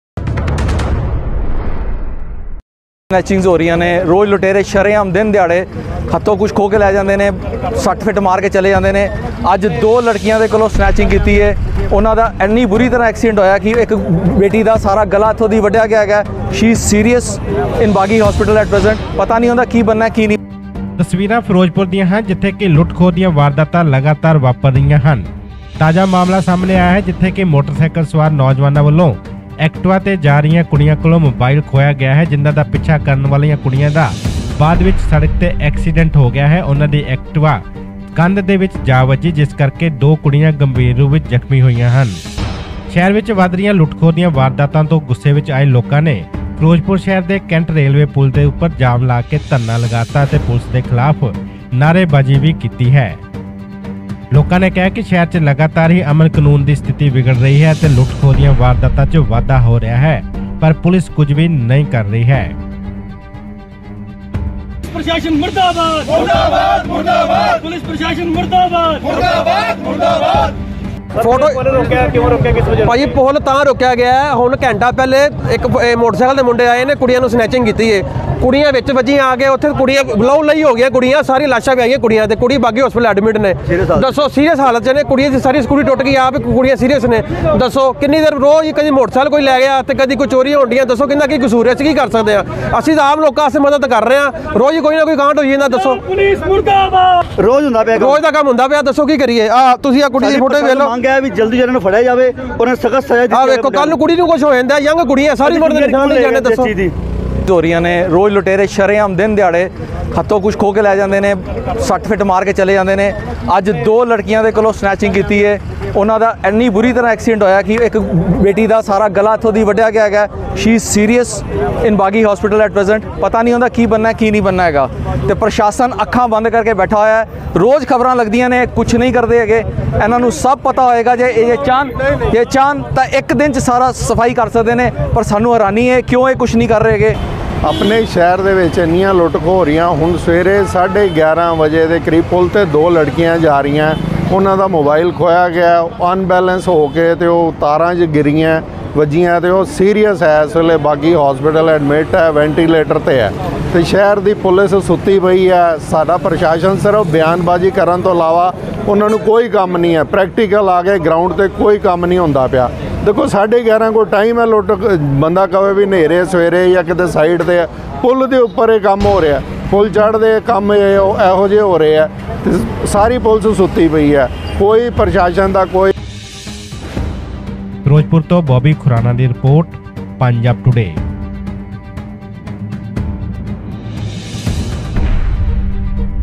Firozpur Snatcher active | Firozpur loot from activa Girls | activa girls accident in firozpur
Today video Firozpur loot from activa girls,  firozpur snatcher mobile loot from girls, activa riding girls accident today firozpur after loot ,
Firozpur public protest against police and snatchers ,

instgrame -@5aabtoday_official